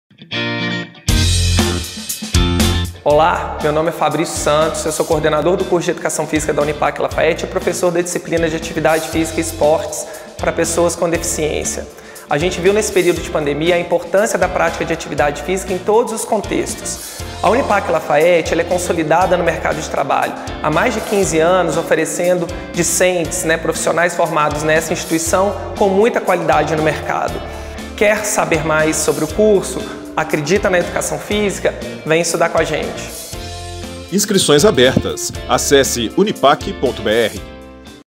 Curso de Educação Física - Vestibular UNIPAC 2021
Você já viu as vantagens de estudar na UNIPAC Lafaiete? O coordenador do curso de Educação Física, Fabrício Santos, veio contar as vantagens de ser um aluno da Instituição.